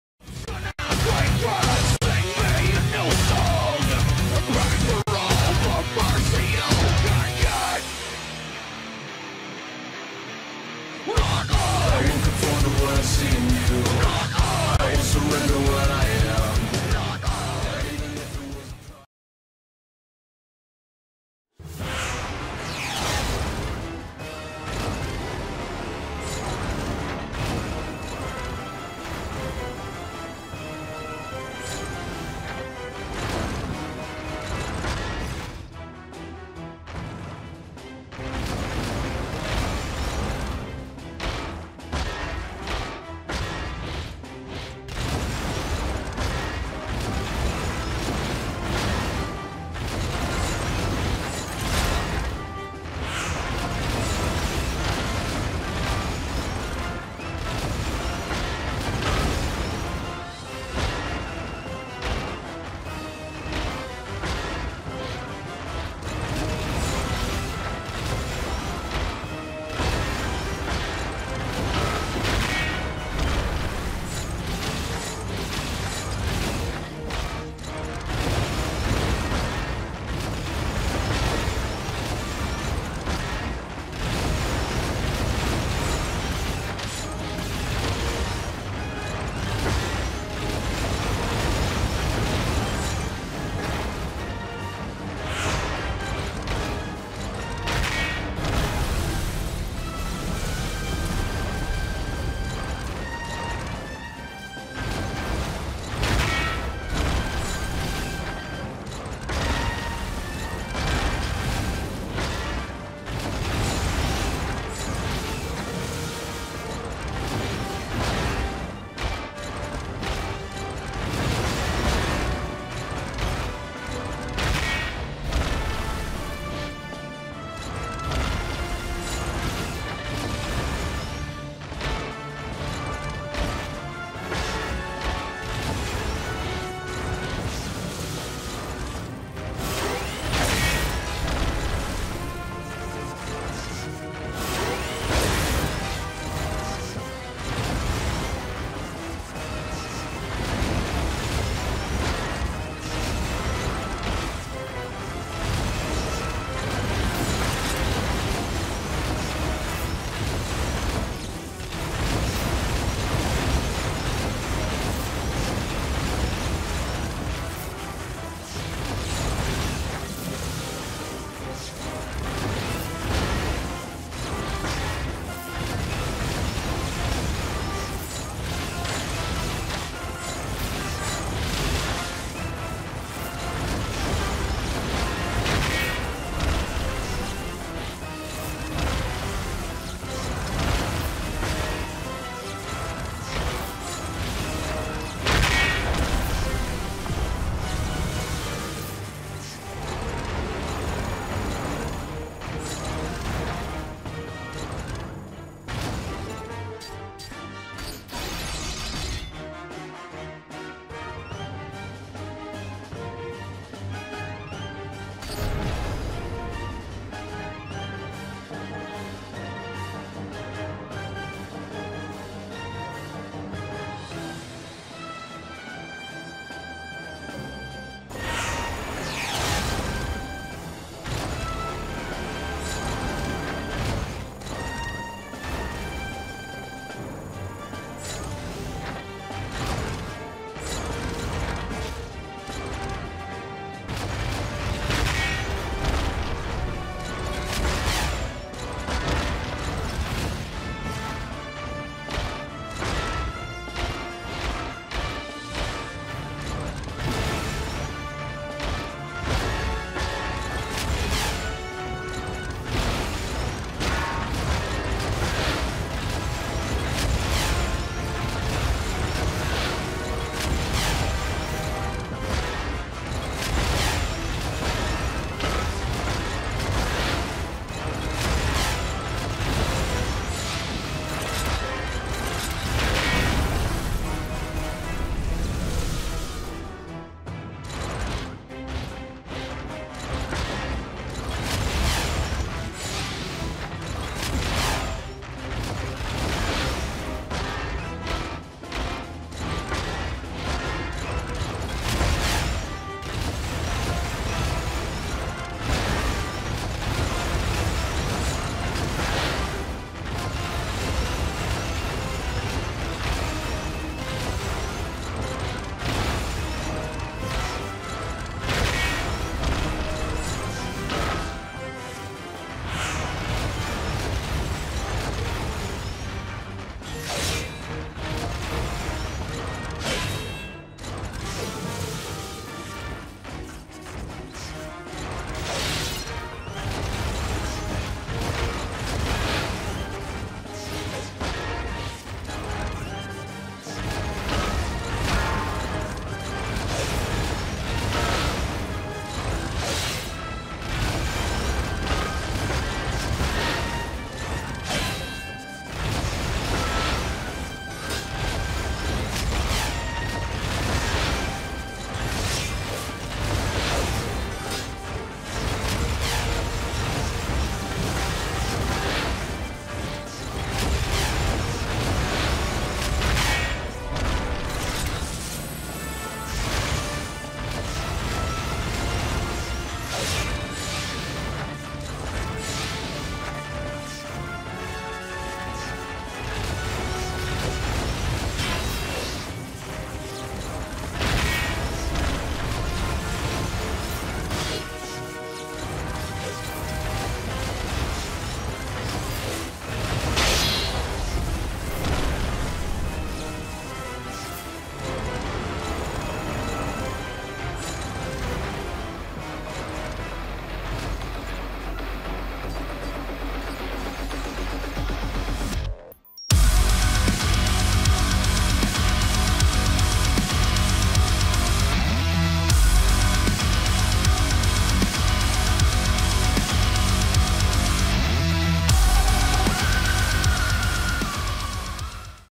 Pacific Rim Battles: Megazord vs. Ignus Faatus
Hey guys, some more Pacific Rim for ya. I'm honestly liking Corion's new design, I think it fits a lot better for the "Tallest Jaeger". I'm also thinking about changing Navy Gypsy's design, perhaps a few alterations or a complete remodel.  If you liked it, make sure to watch the whole video because watch times are IMPORTANT!